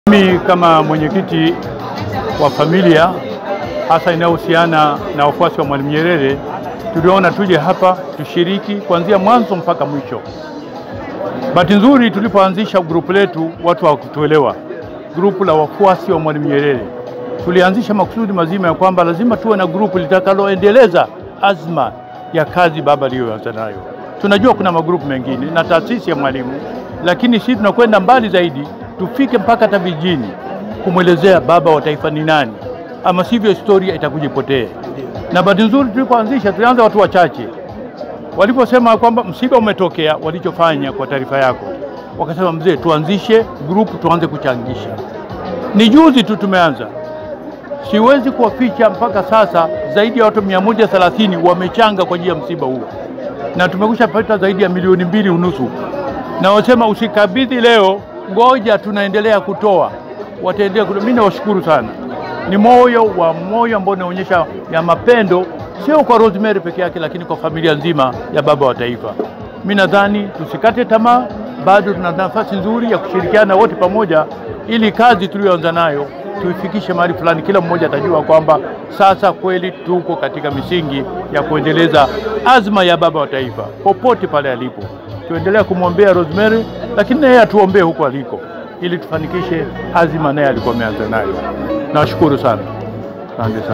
MZEE KIMITI AKIONGEA KWA NIABA YA WANACHAMA WA GROUP LA WAFUASI WA MWALIMU NYERERE.
MZEE PAUL PATER KIMITI AKIWA NA MAKONGORO NYERERE MSIBANI.